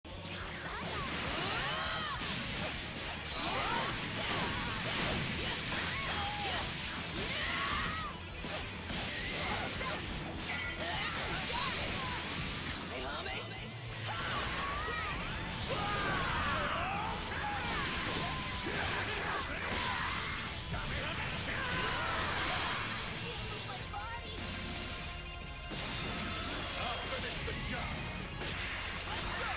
S3 TGohan + Blue Goku + 3rd 214L OOC BGK_B Assist Only ToD
I think someone asked for this but I forget who and admittedly I have been slow with requests lately.

0 bar start (Sparking Lv1)
214L Spark Side Swap 2M 5M JMH(BGK_B) 1S 66 1S 236LMMMM 2M 5HSSSS JLMH J236L Hold (BGK_B*) 214L AA Kamehameha Filler Lv1 Kaioken

* Call 2nd BGK_B Assist halfway into J236L

Seconds as a Corner Route by taking out the side swap.
Using AGohan Lv1 as filler Lv1 because he has the weakest Lv1 DHC in the game.